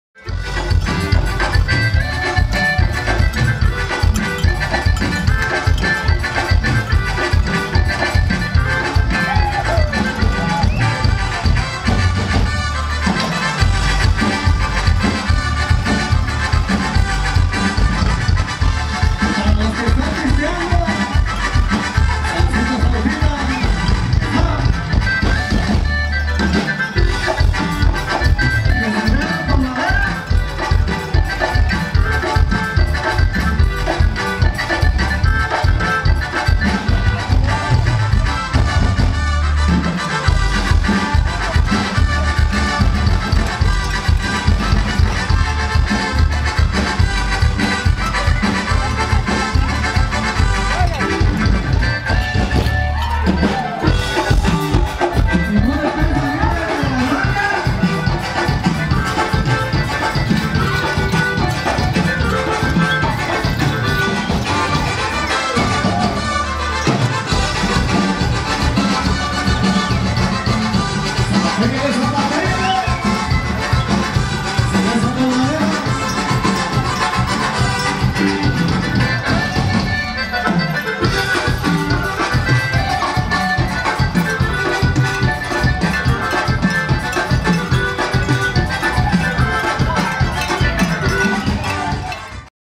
ASI BAILAN LOS CHILOLOS CON LOS COMPAS DE TIERRA MIXTECA
Asi bailan los chilolos de mi lindo San Cristobal Amoltepec Tlaxiaco Oaxaca
Esto fue el cierre del carnaval Seccion Segunda